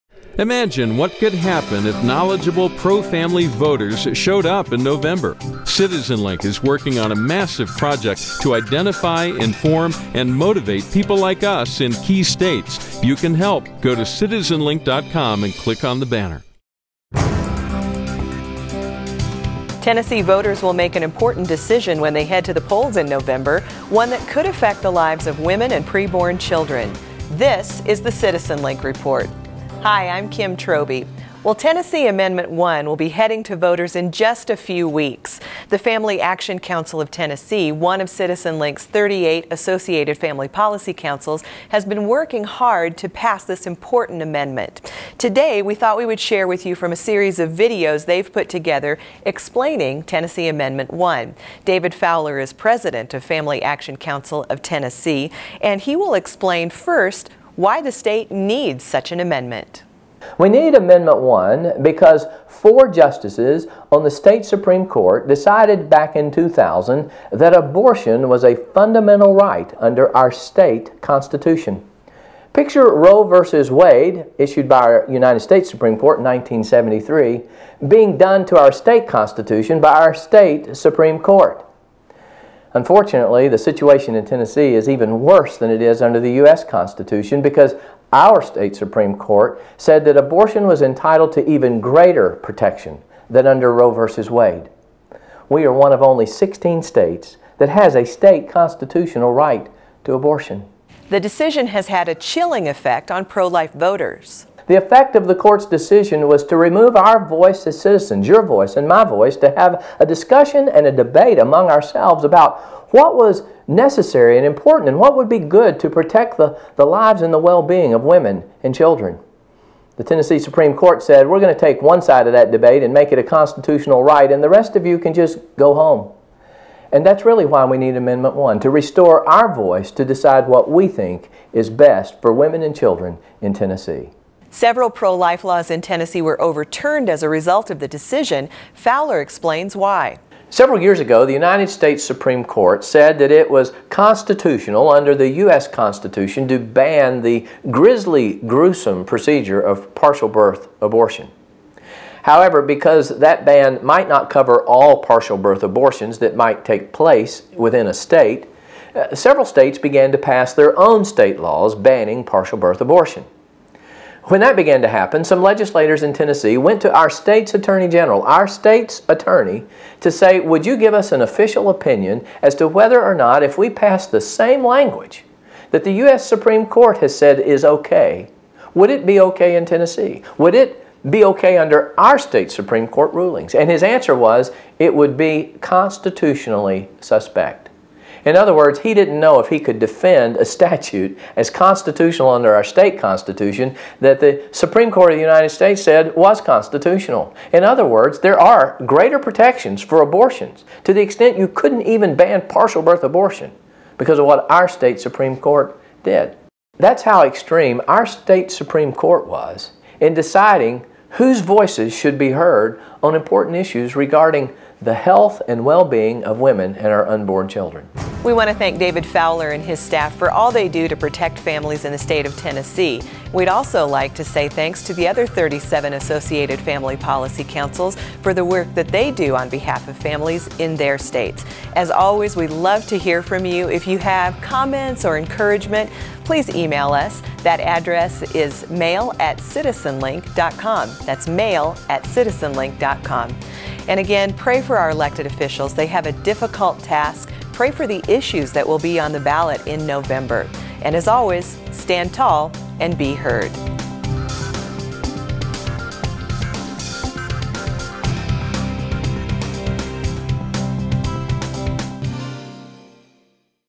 CitizenLink Report: Voters Can Protect Life at the Polls
If Roe v. Wade were overturned today, Tennessee would still have legal abortion. Find out what the Family Action Council of Tennessee is doing to amend the state constitution. David Fowler, the group’s president, talks about the importance of protecting life.